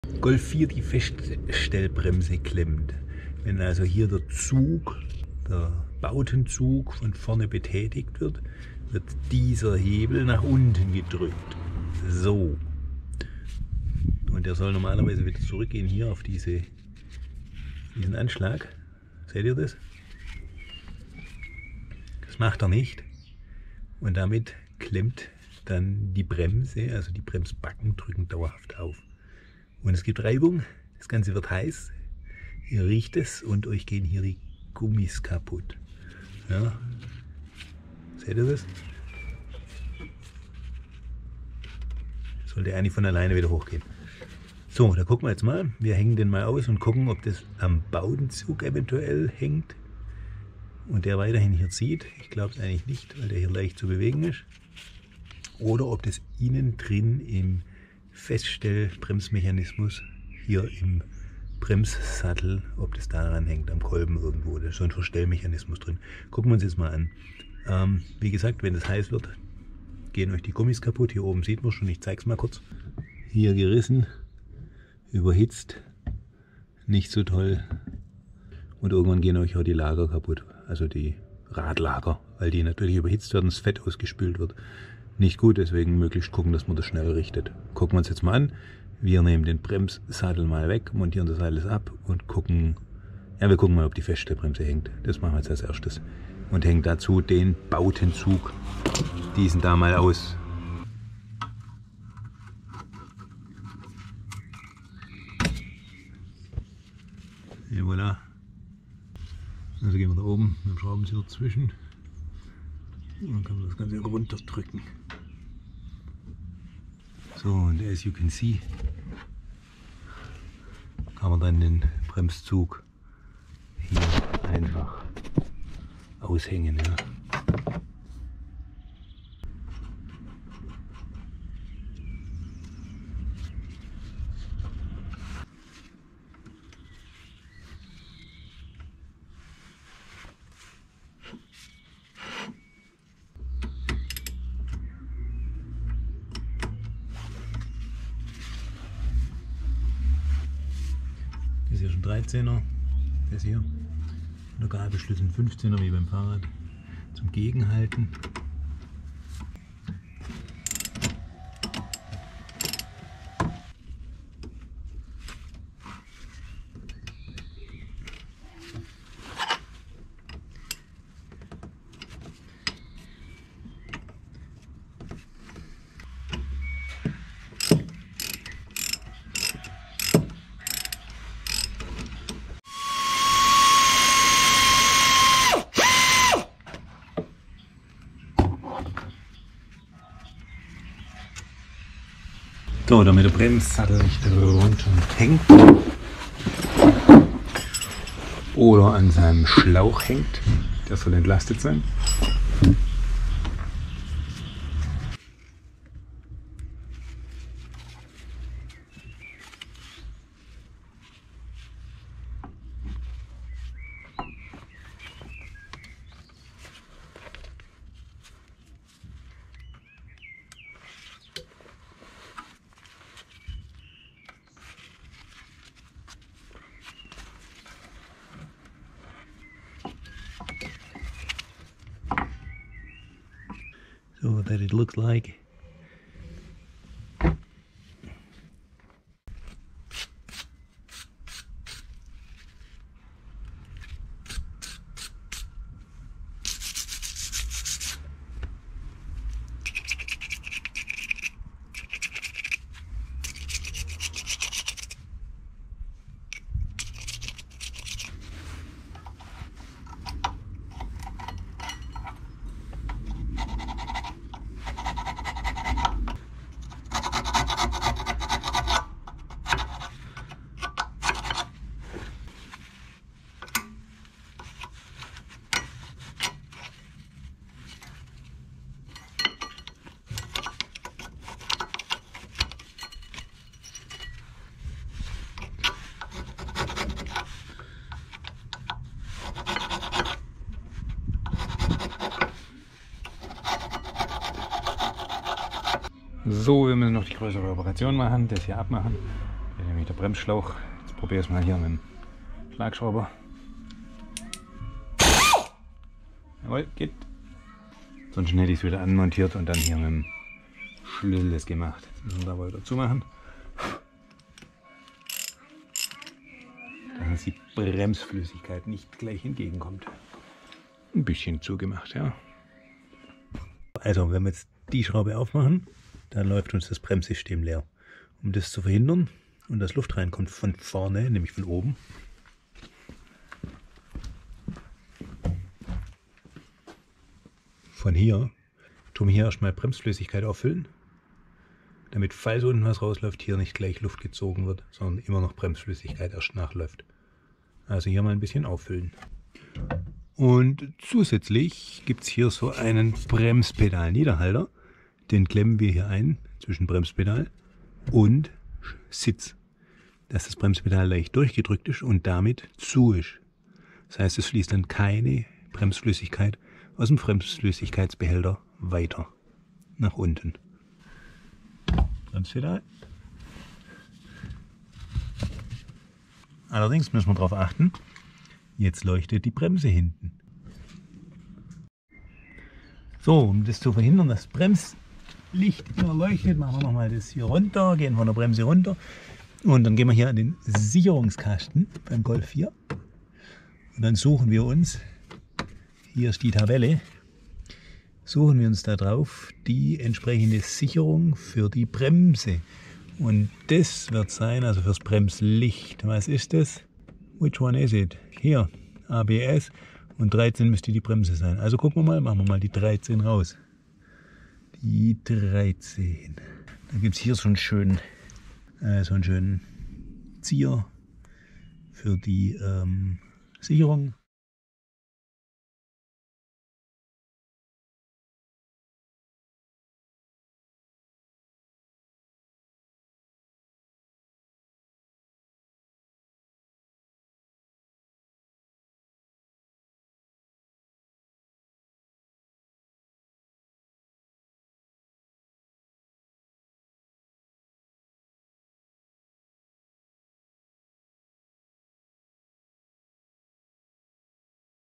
Bremse heiß ??? Feststellbremse defekt???
Welche Symptome eine eine klemmende Bremsmechanik hat, wie lässt sich das feststellen und von klemmenden Bremsseilzügen unterscheiden.

Desweiteren, wie wird der Bremssattel ausgebaut und gereinigt.

00:00 Überblick, Bremshebel geht nicht zurück
01:12 Gummis überhitzt und gerissen
01:42 Bremszug aushängen
02:10 Check ohne Bremszug
02:24 Bremssattel demontieren - Werkzeug
03:12 Bremssattel aufhängen - !!! Bremsschlauch
04:00 Bremsbeläge reinigen - Laufseite beachten
04:18 Führungsflächen der Bremsbeläge reinigen
04:56 Bremsschlauch lösen (Druckluftratsche
05:24 Leerlaufen Bremssystem verhindern - Erklärung
05:36 Bremsflüssigkeitsstand erhöhen
05:49 Bremspedalniederhalter - WIESO WESHALB WARUM
06:23 Sicherung vom Bremslicht ziehen

Viel Spass beim selber machen